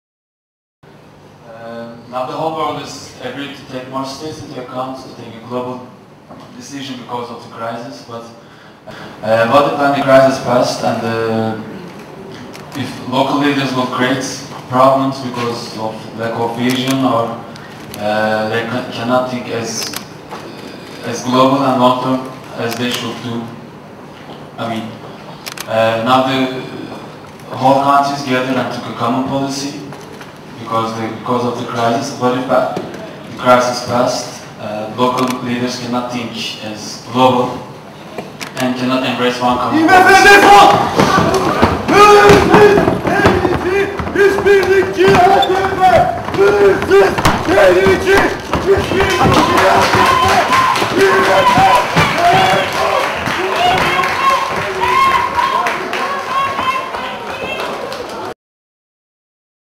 ‘O ayakkabı’ Bilgi’deydi…
Ekim 1, 2009
Uluslararası Para Fonu (IMF) Başkanı Dominique Strauss-Kahn, İstanbul Bilgi Üniversitesi Dolapdere Kampüsü’nde konferans verdiği salonda dinleyiciler arasındaki bir kişi tarafından ayakkabı fırlatılarak protesto edildi.